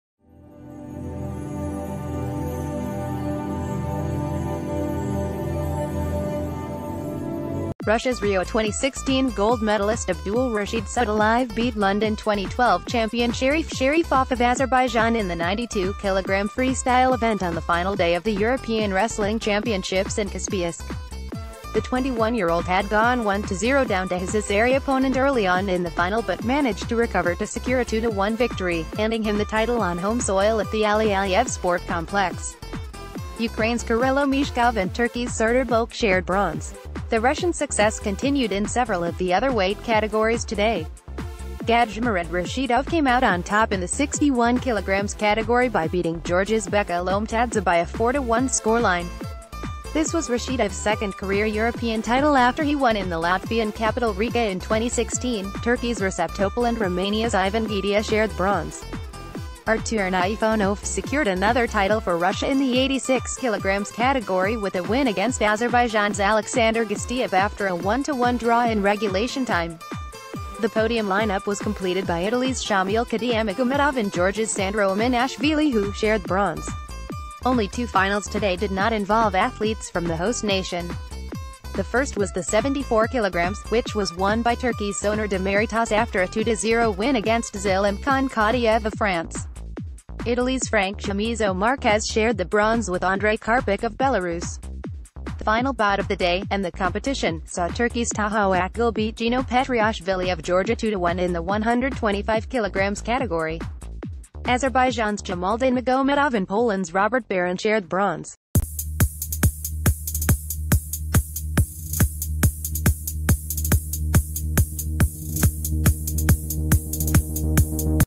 Breaking News | Sadulaev wins battle of Olympic champions at European Wrestling Championships
Russia's Rio 2016 gold medallist Abdulrashid Sadulaev beat London 2012 champion Sharif Sharifov of Azerbaijan in the 92 kilogram freestyle event on the final day of the European Wrestling Championships in Kaspiysk. The 21-year-old had gone 1-0 down to his Azeri opponent early on in the final but managed to recover to secure a 2-1 victory, handing him the title on home soil at the Ali Aliyev Sport Complex. Ukraine's Kyrylo Mieshkov and Turkey's Serdar Boke shared bronze. The Russian success conti...